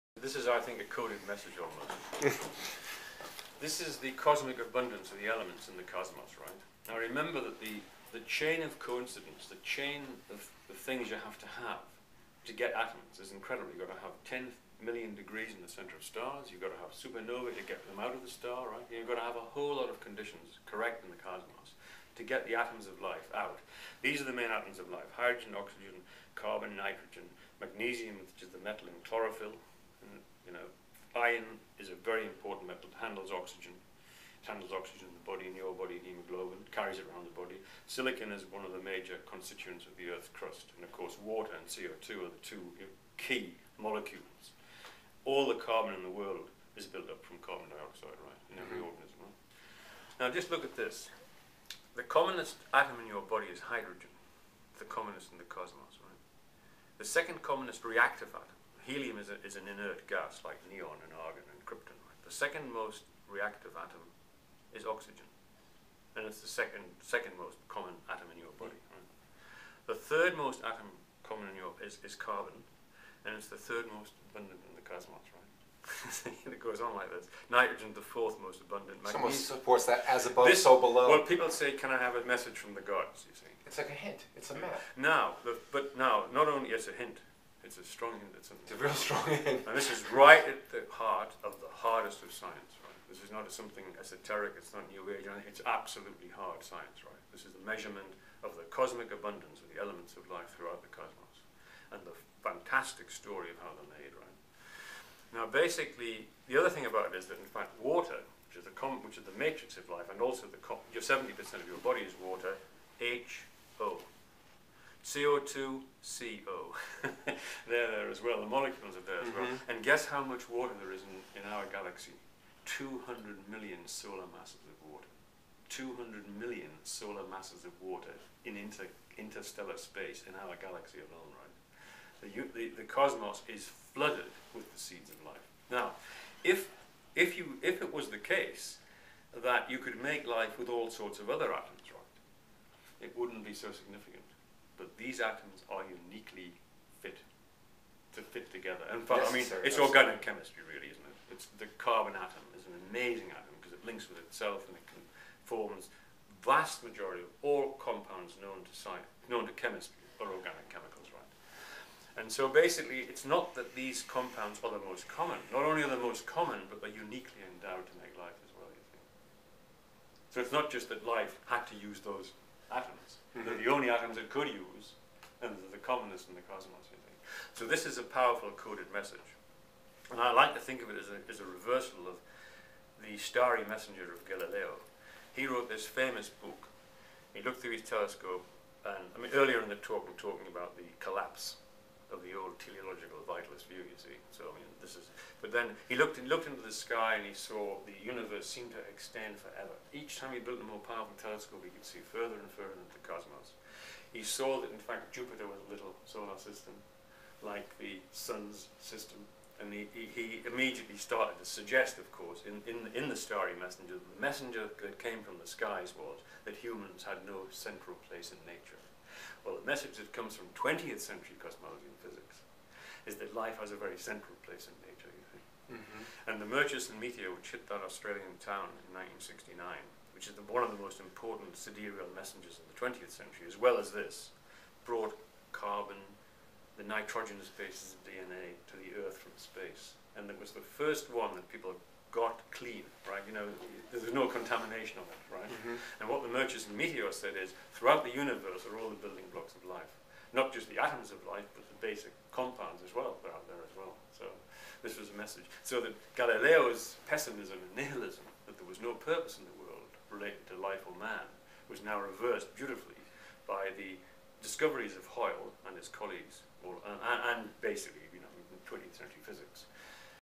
Michael Denton Part 3
Chiropractor Michael Scimeca interviews molecular biologist Michael Denton, M.D., Ph.D. during an impromptu meeting at the April 2009 Vitalism Conference at Life University.